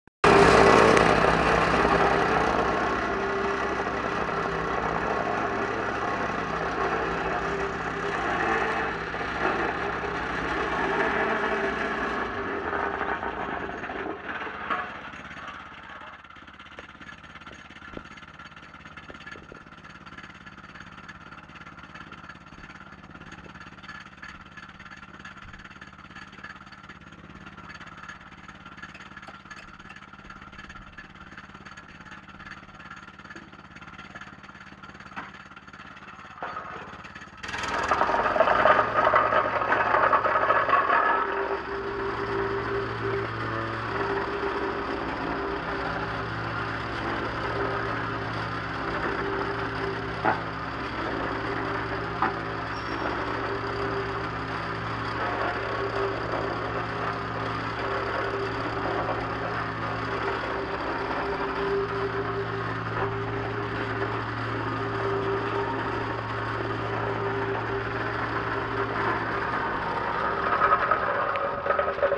Flattening the tarmac! (1)
British Gas sub-contractors working down my road